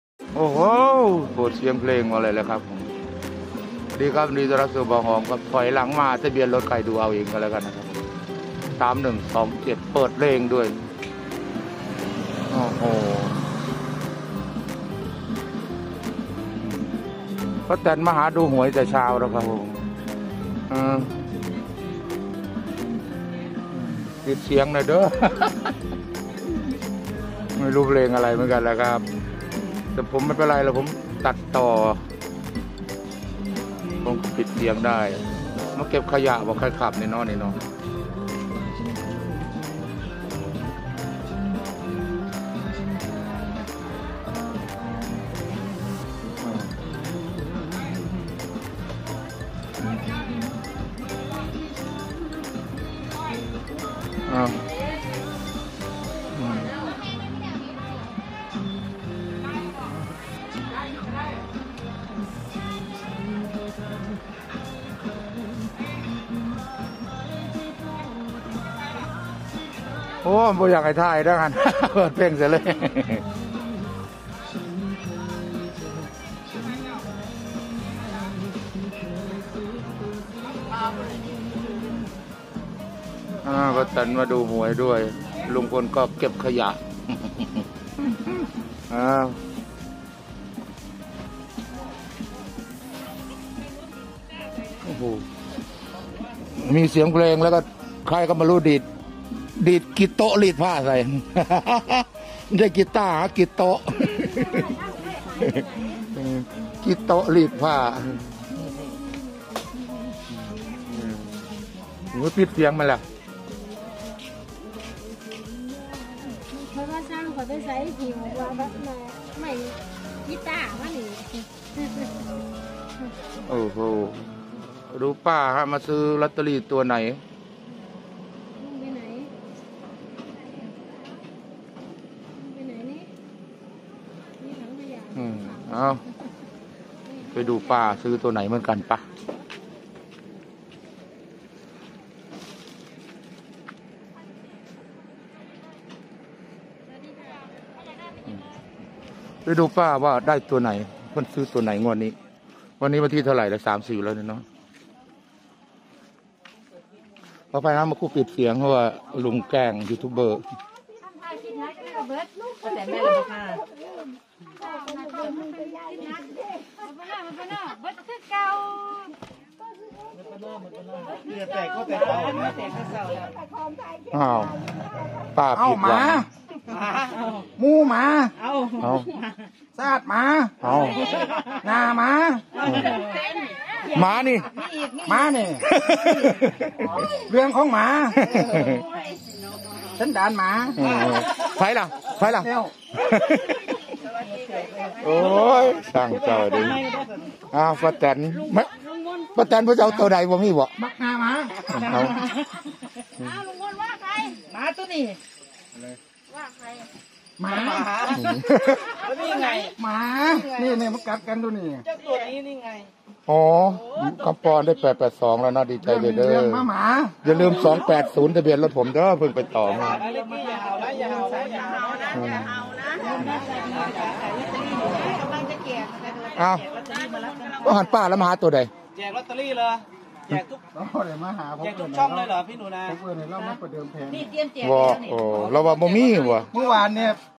เช้าลุงพลไปทิ้งขยะ ป้าแต๋นมาส่องหวย ไปดู
เช้า ลุงพล ไป ทิ้งขยะ ป้าแต๋น มาส่องหวย ไปดู